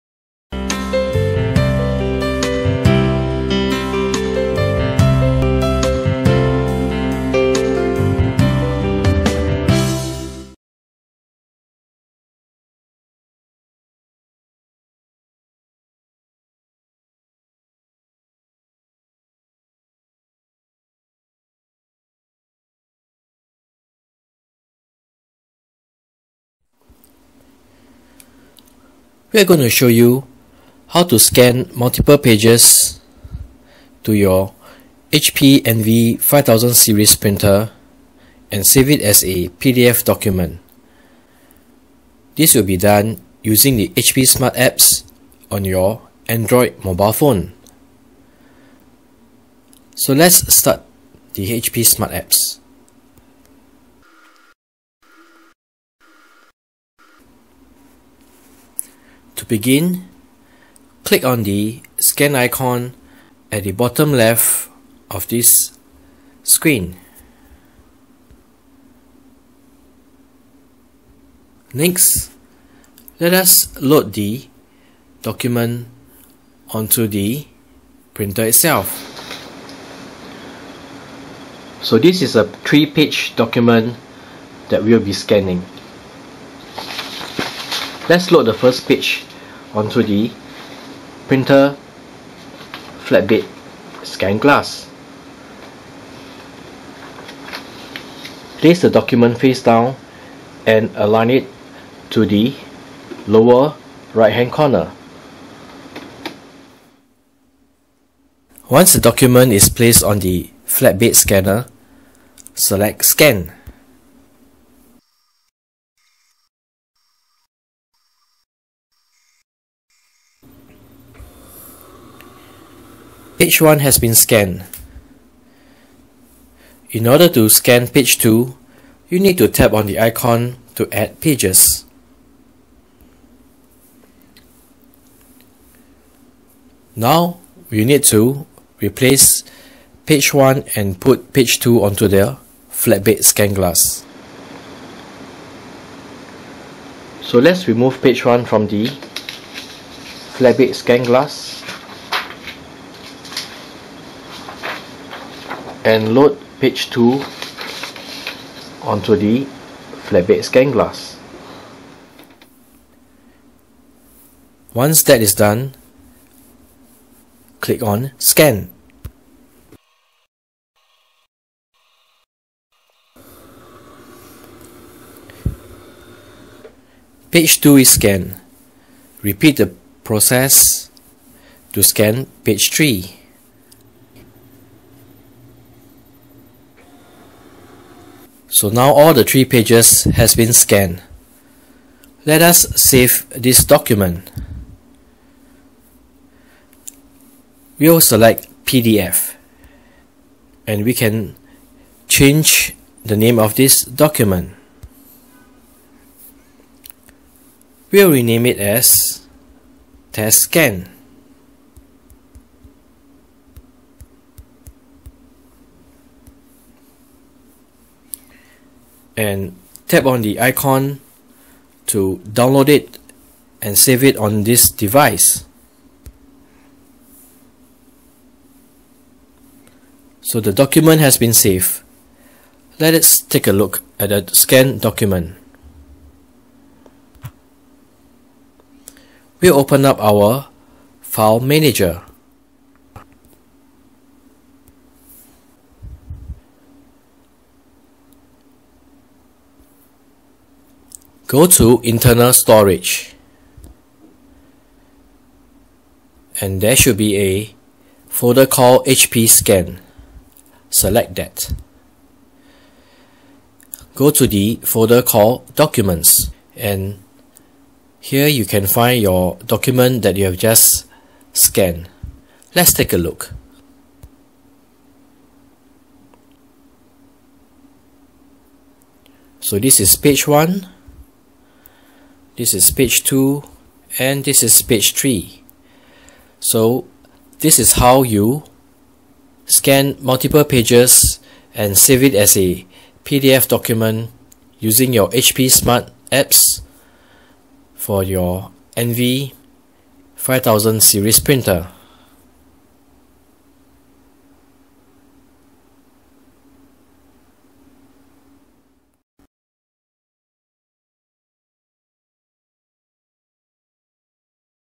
HP Envy 5020 | 5030 | 5055 : Scan multiple pages and save as a PDF document using HP Smart
This tutorial shows how to use the HP Smart apps to scan multiple
pages & save as a PDF document.

The printer needs to be connected to the network first with the HP Smart apps installed on the PC or phone.

This is applicable on the HP OfficeJet 5200, HP Envy Photo 6200/7100/7800 and HP Envy 5000 AiO series printers.